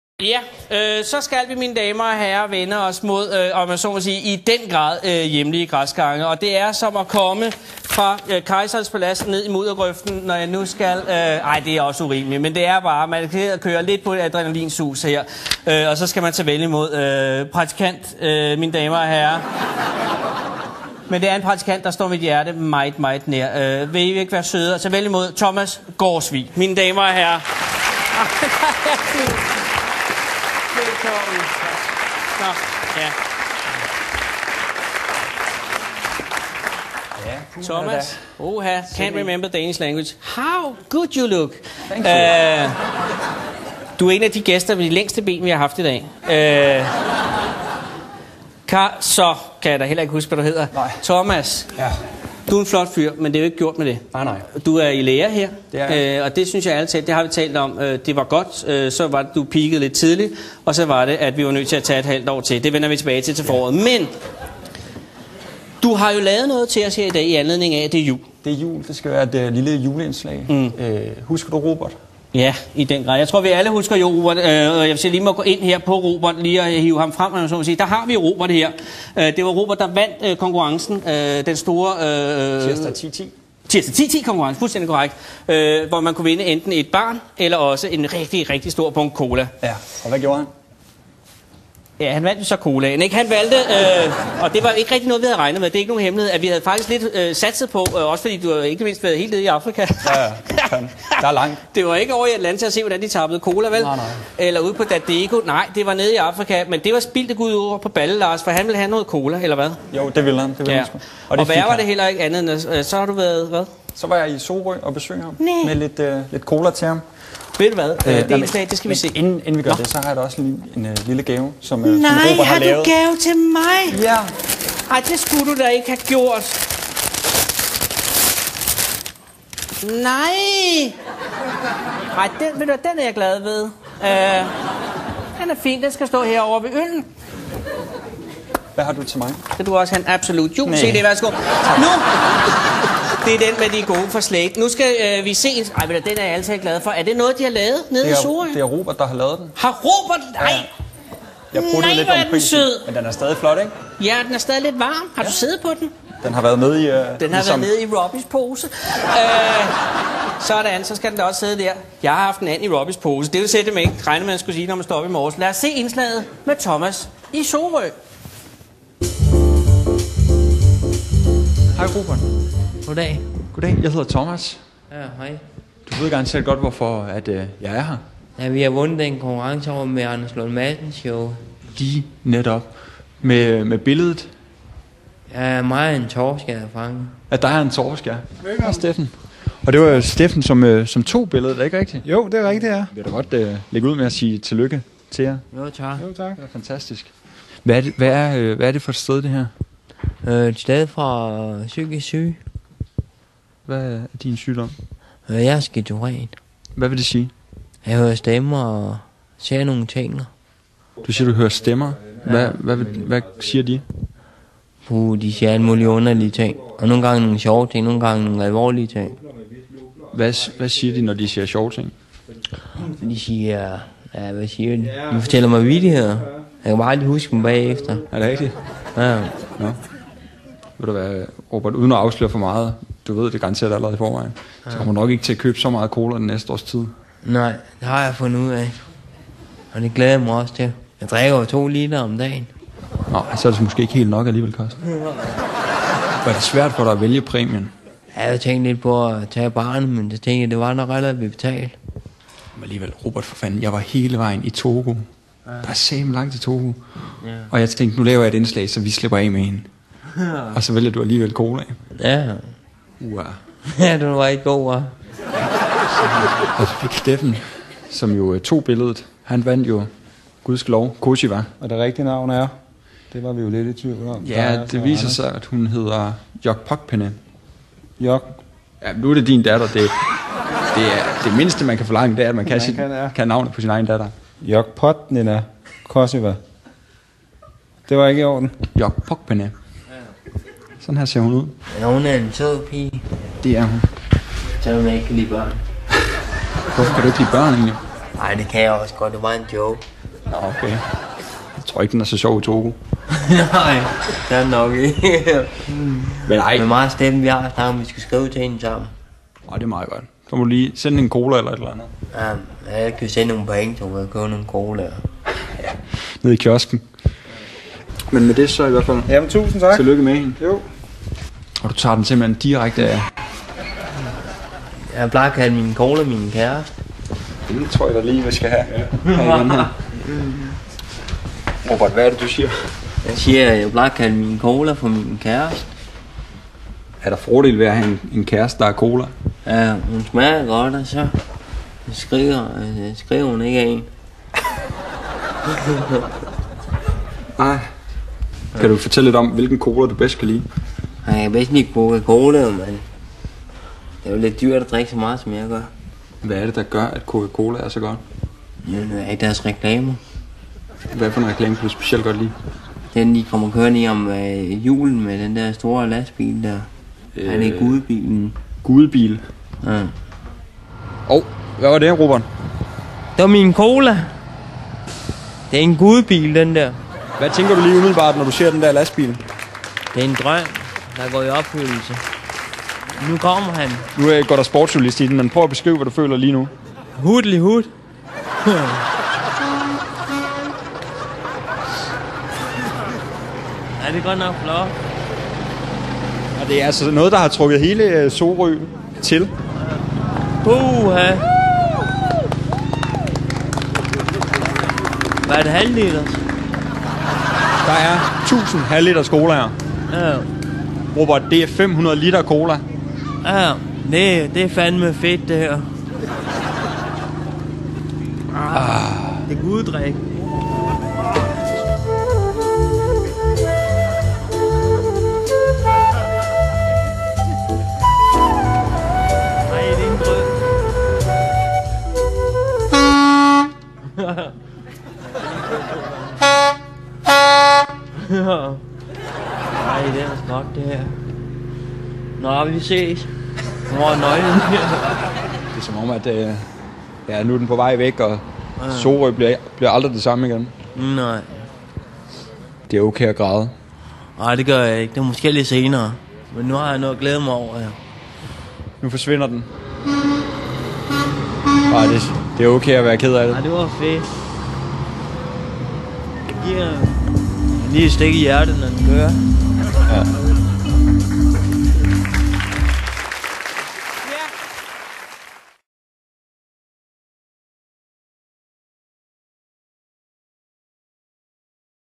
Praktikanten
Praktikanten Giver "Robert" et halvt års forbrug af coca cola. Manden bliver fuldstændig lykkelig. vi kan vidst hurtigt blive enige - cola er bedre end en baby!